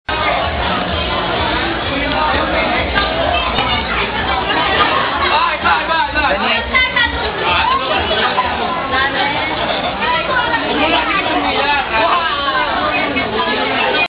Subic Bay Travelers Hotel on February 14, 2012 (Part 54)
The Junior-Senior Promenade was held at Subic Bay Travelers Hotel. Joshua David (III-Galileo) and Denise Gamboa (III-Euclid) crowned as Prom Prince & Princess, respectively, while Migs Simsuangco (IV-Cobalt) and Rausche Sausa (IV-Cobalt) crowned as Prom King & Queen, respectively.